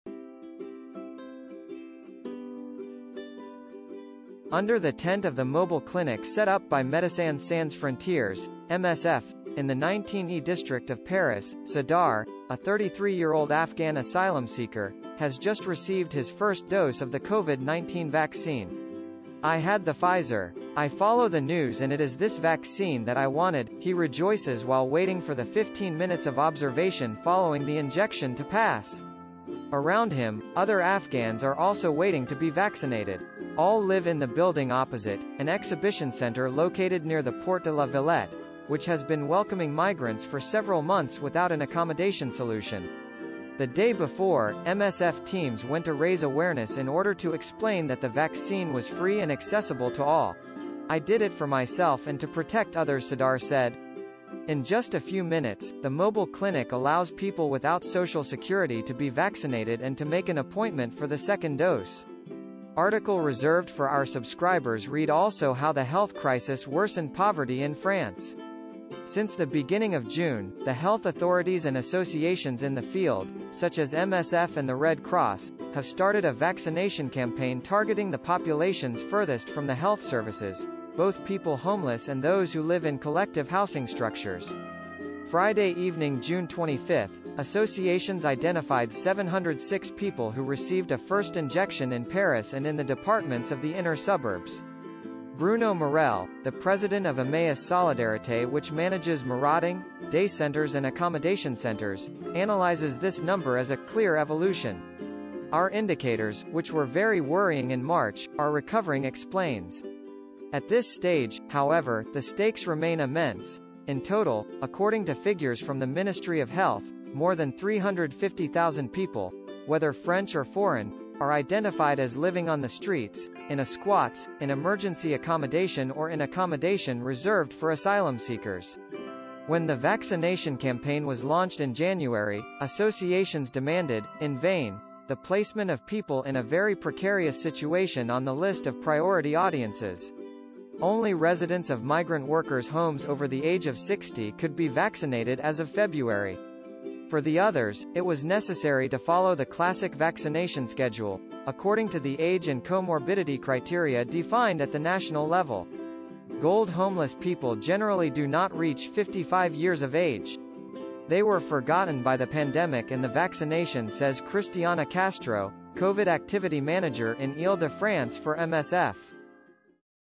Covid-19: people in great precariousness “have been forgotten by vaccination”
Under the tent of the mobile clinic set up by Médecins sans frontières (MSF) in the 19e district of Paris, Sadar, a 33-year-old Afghan asylum seeker, has just received his first dose of the Covid-19 vaccine. “I had the Pfizer. I follow the news and it is this vaccine that I wanted ”, he rejoices while waiting for the fifteen minutes of observation following the injection to pass. Around him, other Afghans are also waiting to be vaccinated.

All live in the building opposite, an exhibition center located near the Porte de la Villette, which has been welcoming migrants for several months without an accommodation solution. The day before, MSF teams went to raise awareness in order to explain that the vaccine was free and accessible to all. “I did it for myself and to protect others”, Sadar said. In just a few minutes, the mobile clinic allows people without social security to be vaccinated and to make an appointment for the second dose.